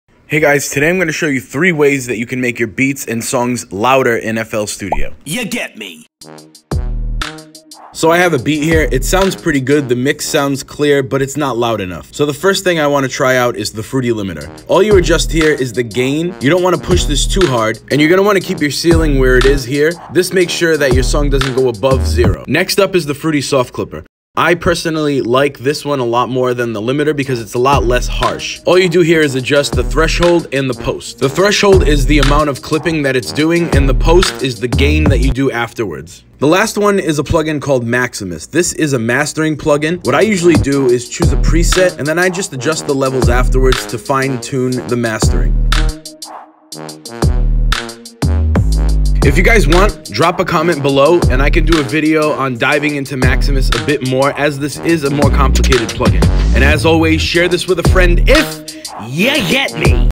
3 Ways to Make Your Beats Louder in FL Studio
Here’s my top 3 ways to make your beats and songs louder with FL Studio native plugins!